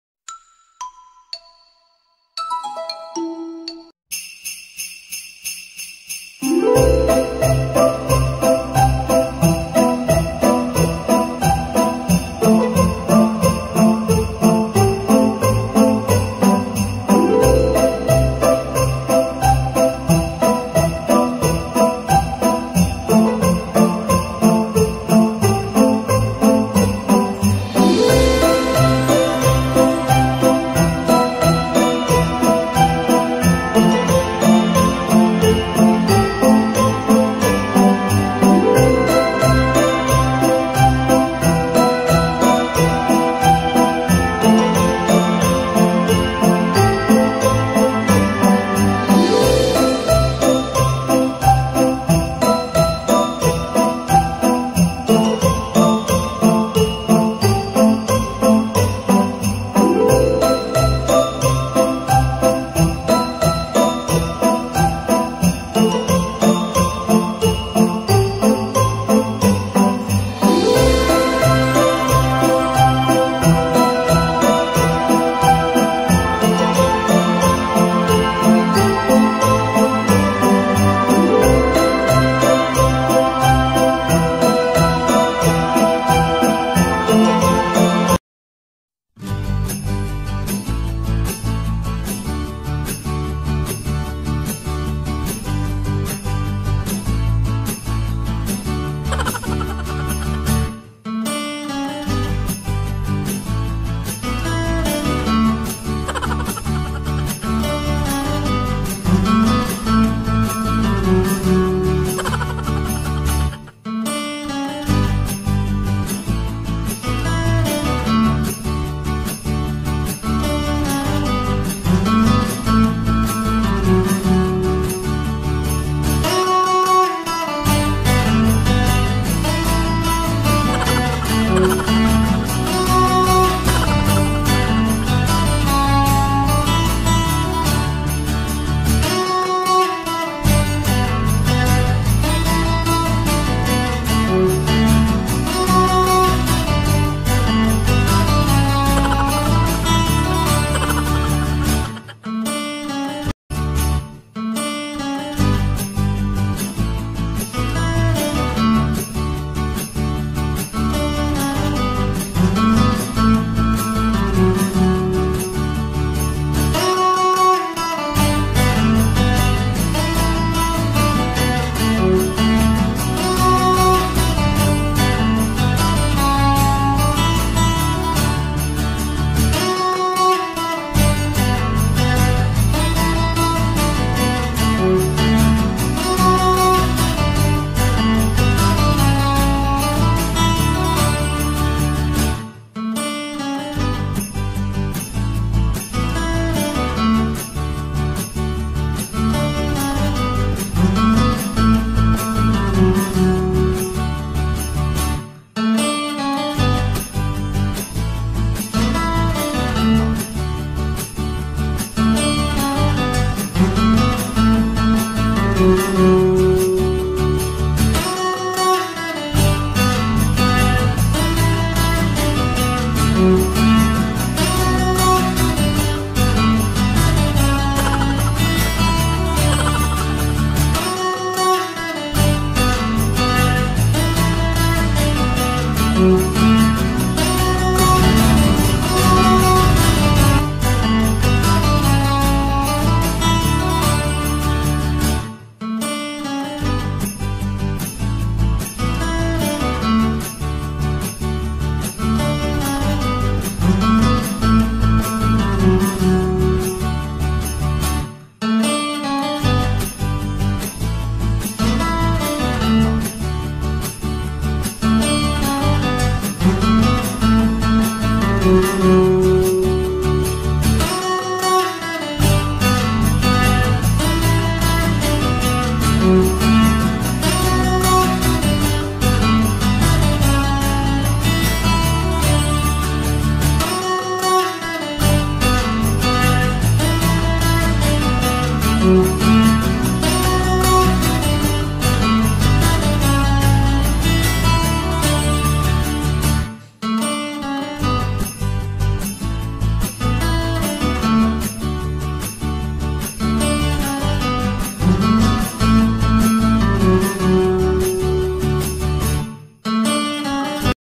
NEW YEAR CELEBRATION 2020 | LATE UPLOAD | GRASYA'S VLOG!
Hello everyone, alam ko late upload na ang new year celebration namin ng aking family pero sana ma-enjoy niyo pa din kasi masaya at puro kakwelahan ang ganap! Thank you and godbless everyone! Stay positive! Keep faith! Kahet ano pang problema mo! Kaya mo  yan! -grasya❤️